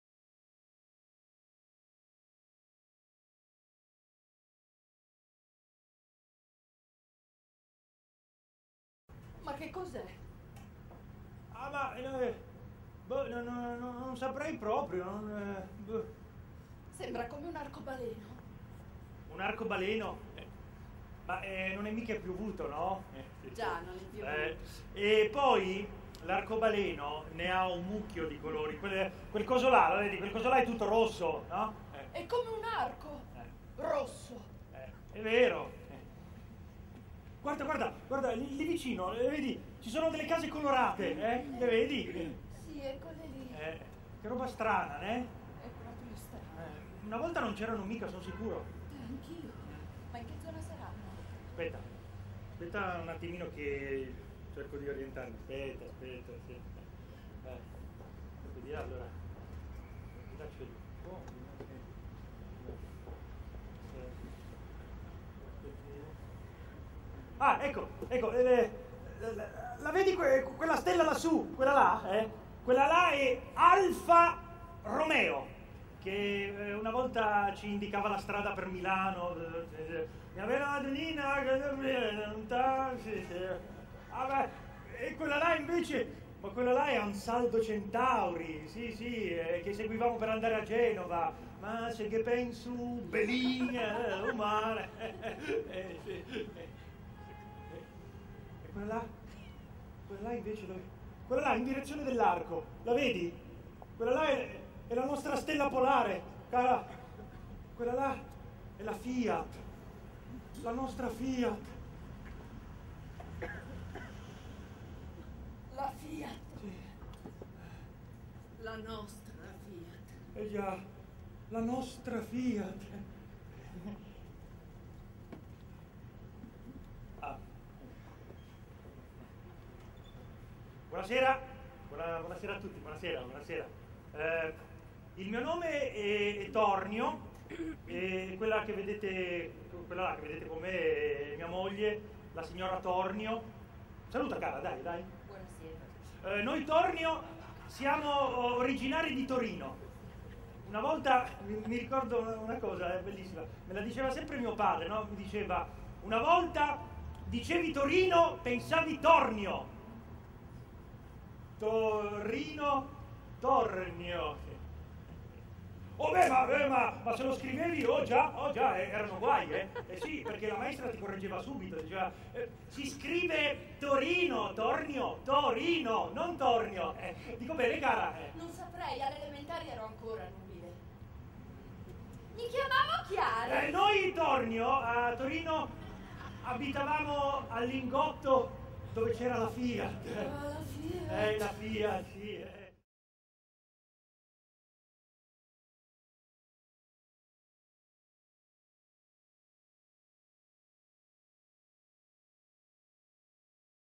Ritorno a Torino dei signori Tornio - Trailer
atto unico di Giuseppe Culicchia
da un’idea di Luciano Filicetti

Regia di Mauro Avogadro

con Gianluca Gambino, Lorenzo Iacona, Irene Ivaldi, Rossana Mortara, Roberta Calia, Ivana Cravero, Pablo Gaston Franchini, Diego Iannaccone, Marlen Pizzo, Francesca Simonetti, Angelo Tronca.
una produzione del Teatro Stabile di Torino in collaborazione con GTT (Gruppo Torinese Trasporti)

Un tram storico riportato all’antico splendore diventa palco itinerante per le vie di Torino.
Torino, Tram storico (29 settembre 2007: Giardini Reali; 30 settembre 2007: Gran Madre; 5 ottobre 2007: deposito GTT San Paolo, via Monginevro; 6 ottobre 2007: corso Marconi / Via Madama Cristina; 7 ottobre 2007: Falchera, capolinea linea 4)